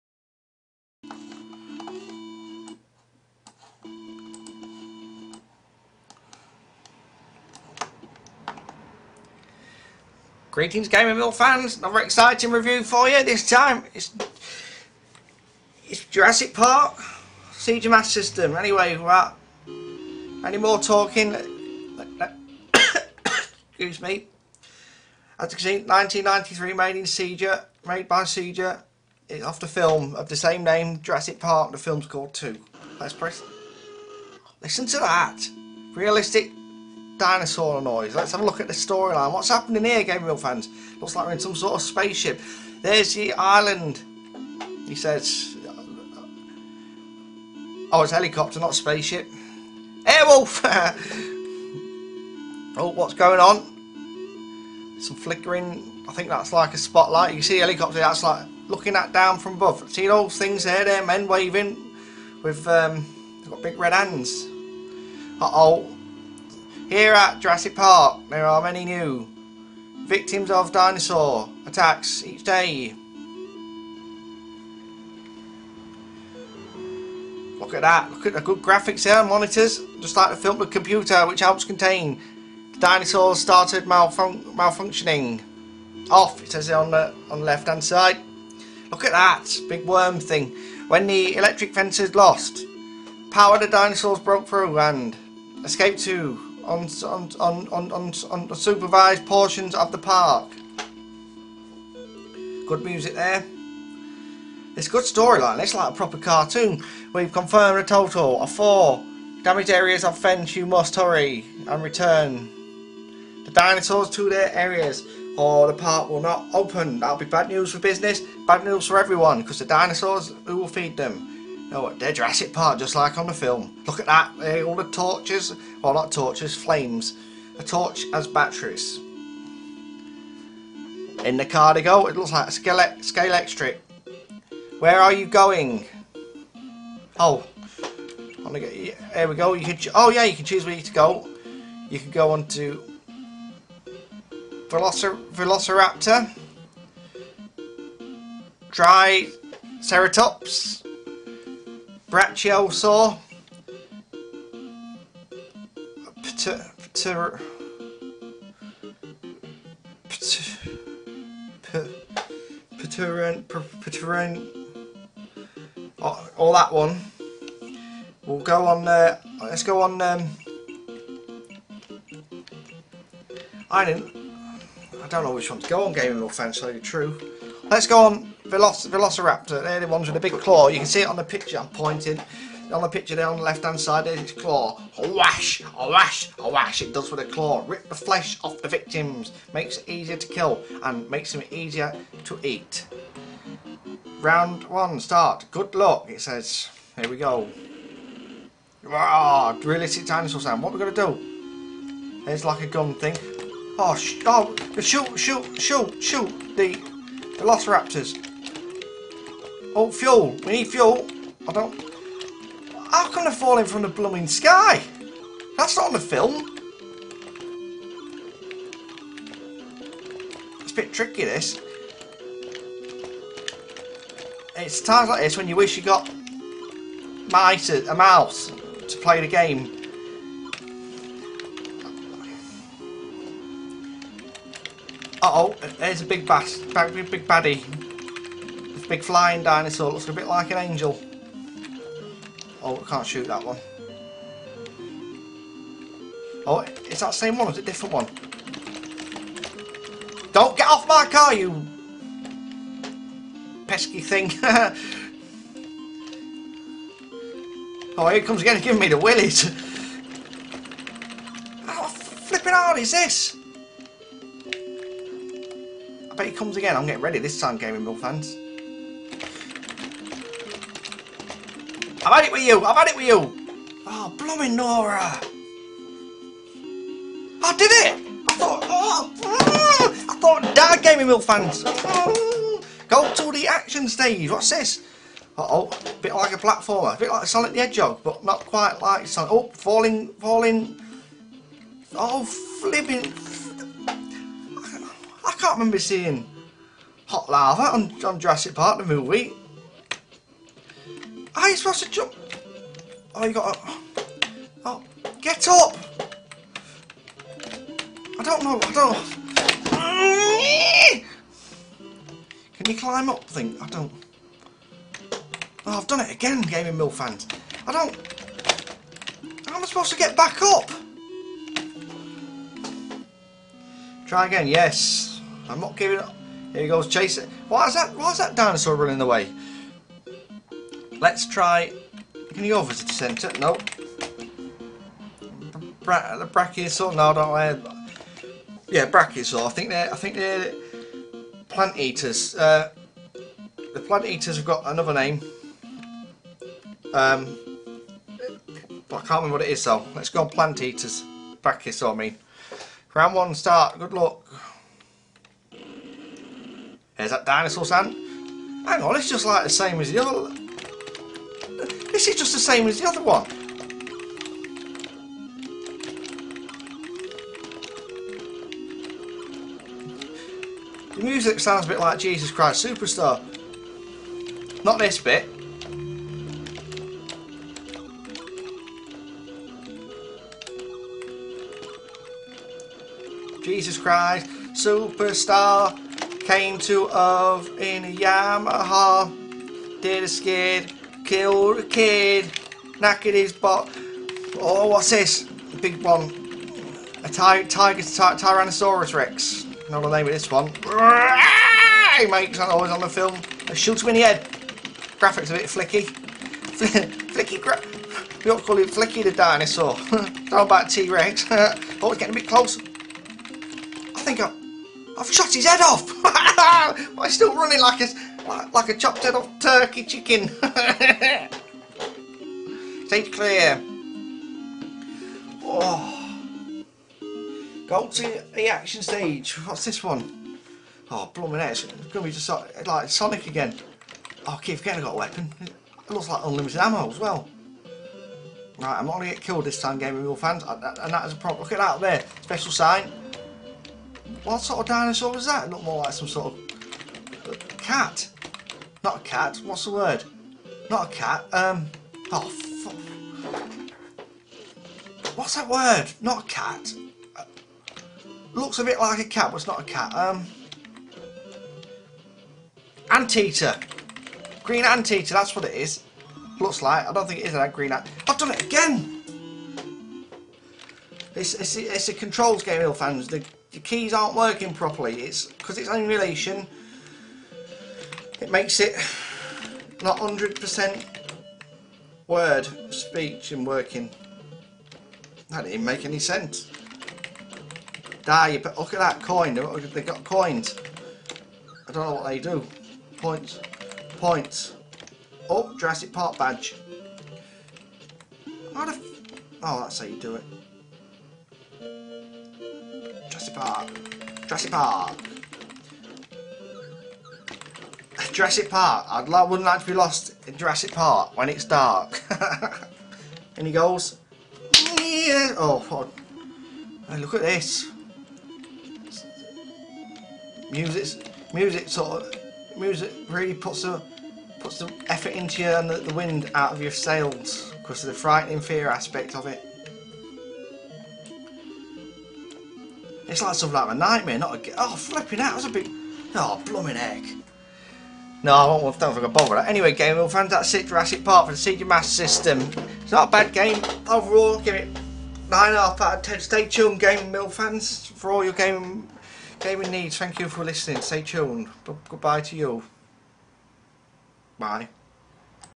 Jurassic Park review on the Sega Master System!!!
He we go - Jurassic Park review on the Sega Master System! It's like the film by Steven The Speilberg! YAS!!!
Keep your cotton buds dry!

WONGA!!!